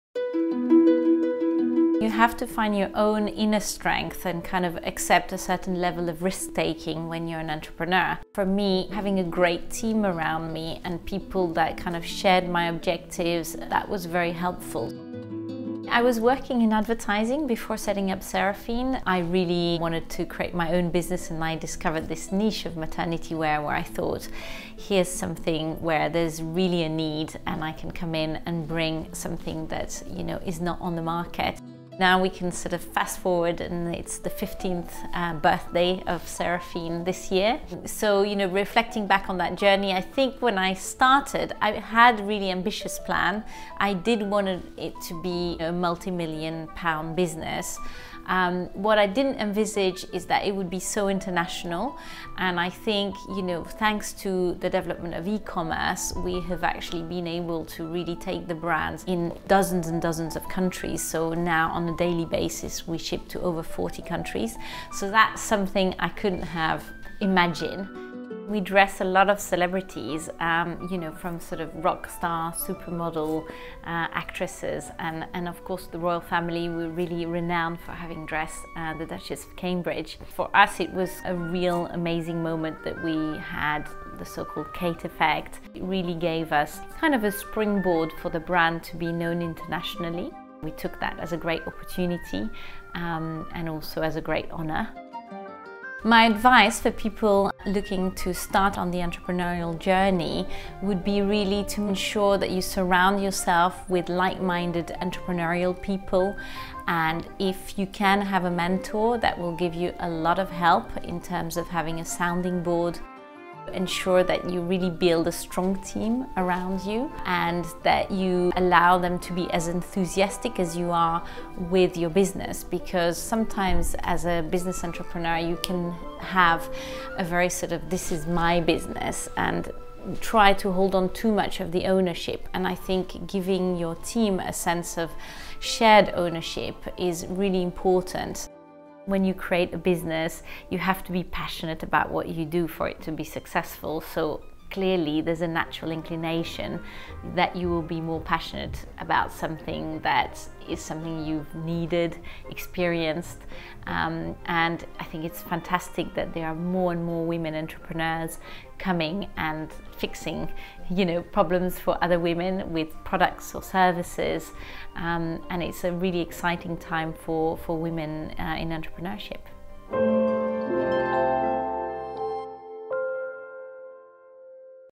Cecile Reinaud, PwC '5 days of women' 2017
Cecile Reinaud, founder and CEO of Seraphine shares her advice for budding entrepreneurs and why she believes it's an exciting time for women in entrepreneurship as part of our 5 Days of Women campaign.